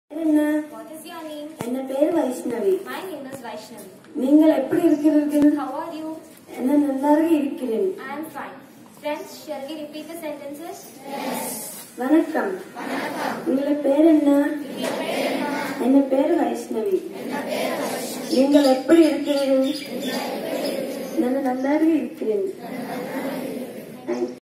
Tamil Language - Bhasha Sangam - Kendriya Vidyalaya Guntur Shift-1
Language: TAMIL
Name of the School: Kendriya Vidyalaya Guntur (Shift-1)
Full address of the school :  Kendriya Vidyalaya Nallapadu, Guntur , Andhra Pradesh - 522005
Details of the Video: Students learning TAMIL Language in morning assembly